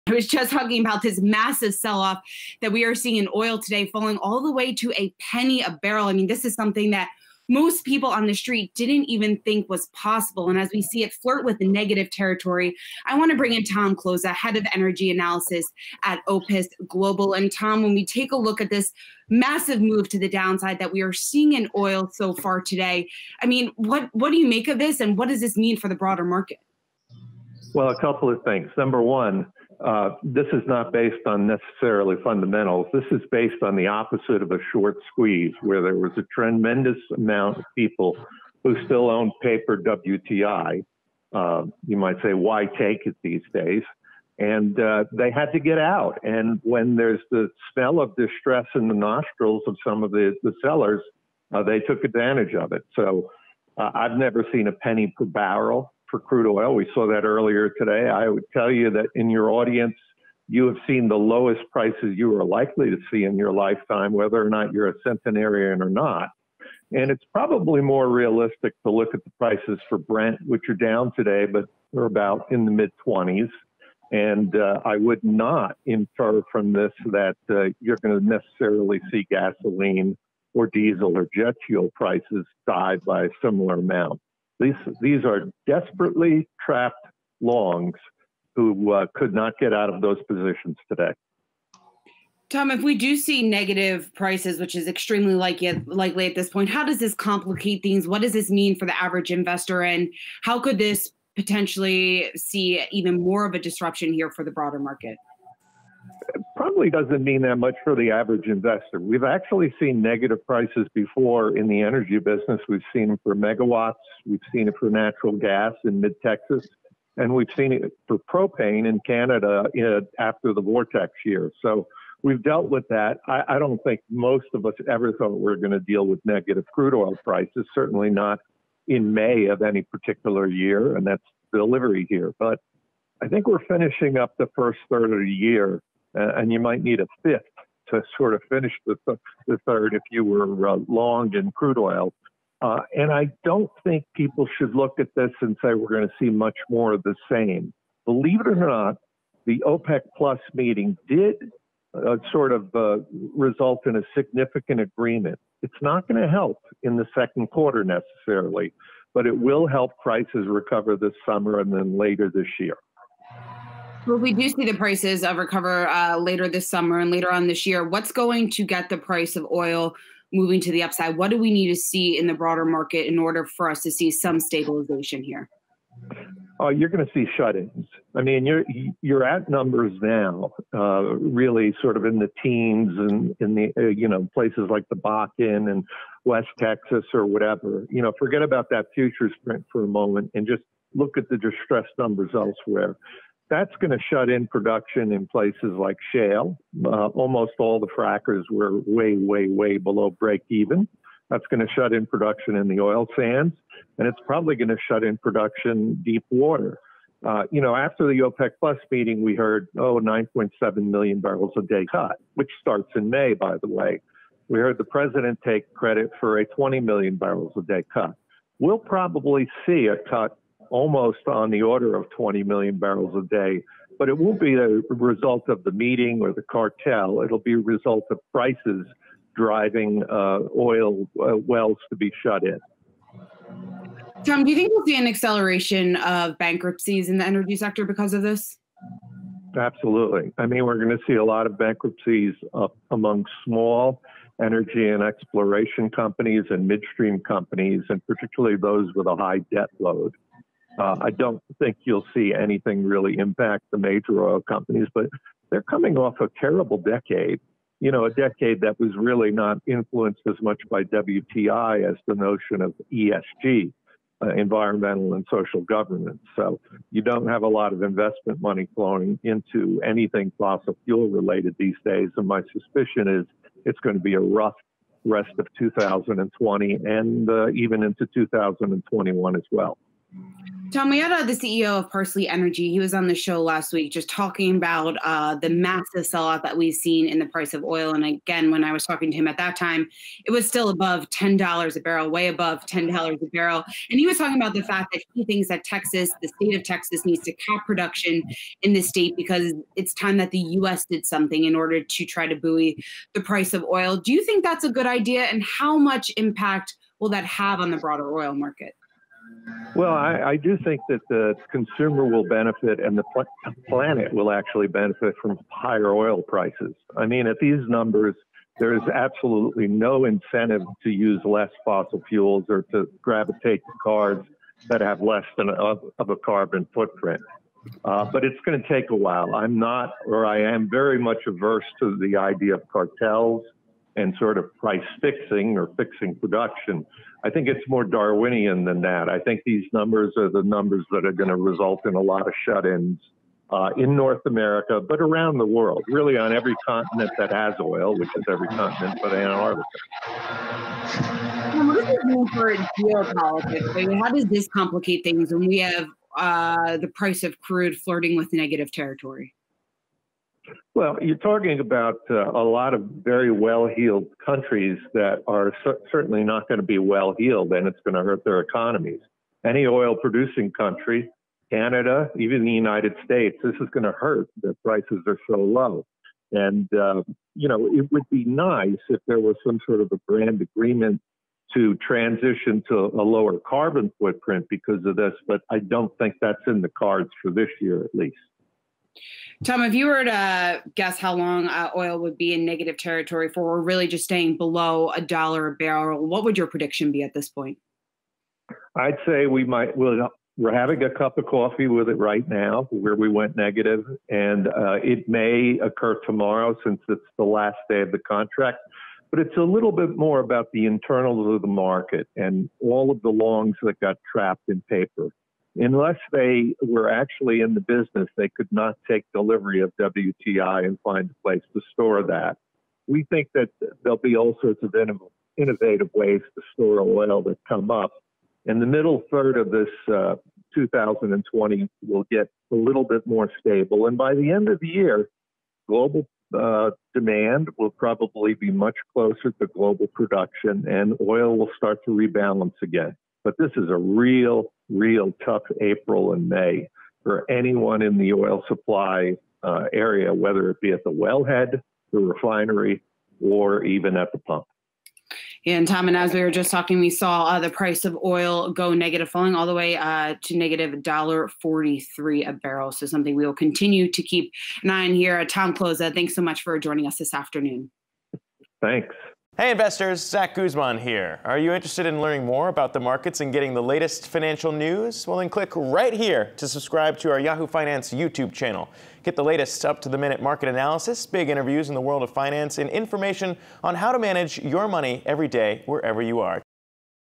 WTI crude oil futures collapse to negative price for first time in history
OPIS Global Head of Energy Analysis Tom Kloza joins Yahoo Finance’s Seana Smith to break down the latest energy news, as crude oil falls into negative territory.